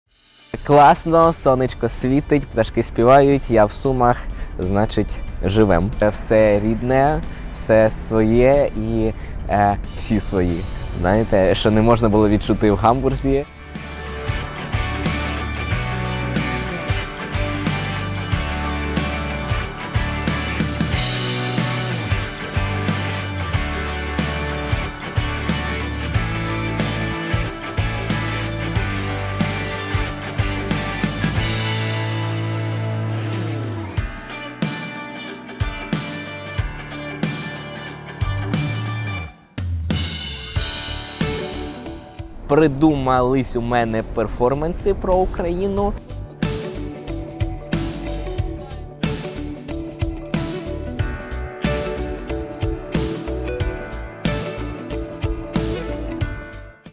Повернувся з Німеччини в Суми: історія 17-річного Нікіти Семи
Повернення додому після двох років у Німеччині – 17-річний сумчанин Нікіта Сема разом з родиною приїхав в Суми. Перебуваючи за кордоном, юнак розповідав німцям про українське життя під обстрілами, а також збирав кошти на безпілотники військовим. Чому вирішив повернутися саме зараз та чи бачить своє майбутнє у прикордонних Сумах – розповів кореспондентам Суспільного.